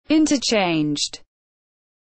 How to pronounce Interchanged / Interchanged pronunciation
Raccoon teaches you how to pronounce Interchanged . Raccoon vous apprends a prononcer Interchanged . Como pronunciar Interchanged